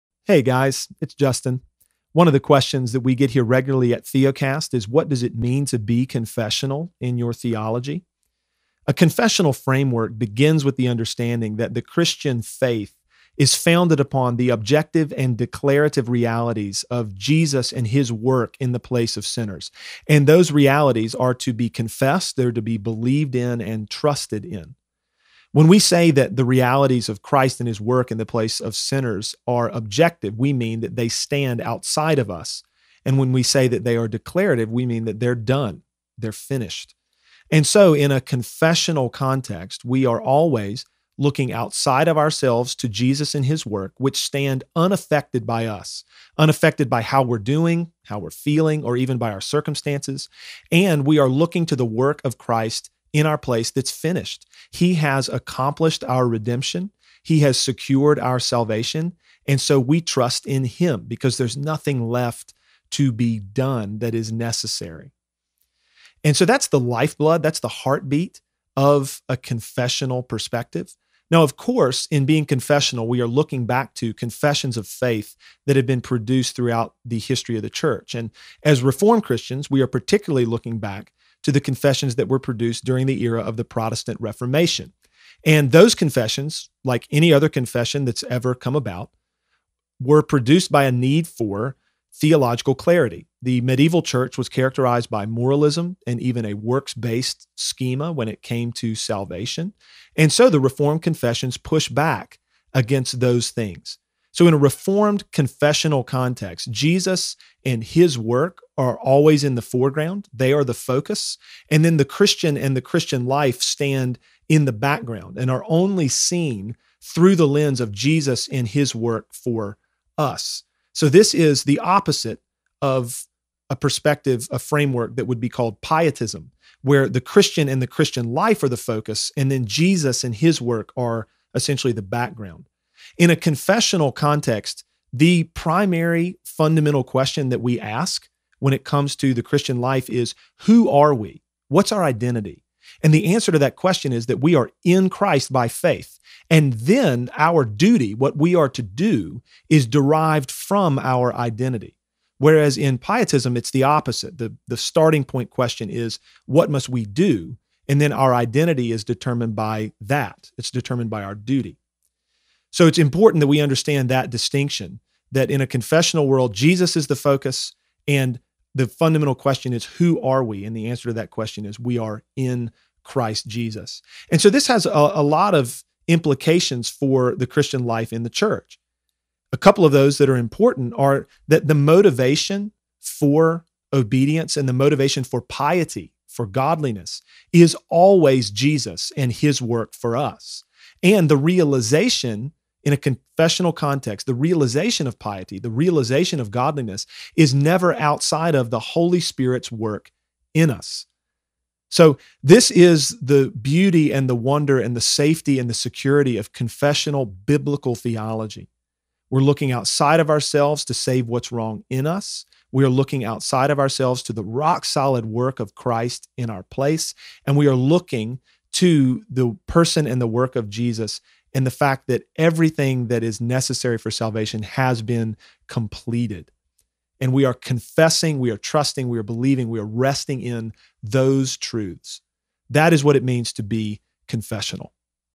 What Does It Mean To Be Confessional? | askTheocast.com
What Does It Mean to Be Confessional? Answered by Justin Perdue

Ask your question at askTheocast.com